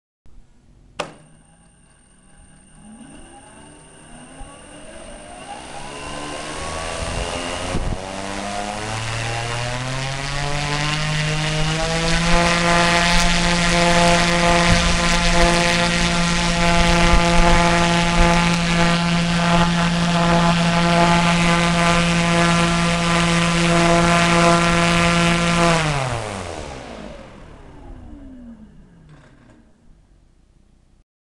Full test CA80-80 on 6S
Prop 25x12 beech wood Fiala Propellers, Bat Zippy 6S 5Ah full charged, Trust 7,7kg, Current from Bat 54A, Voltage 22V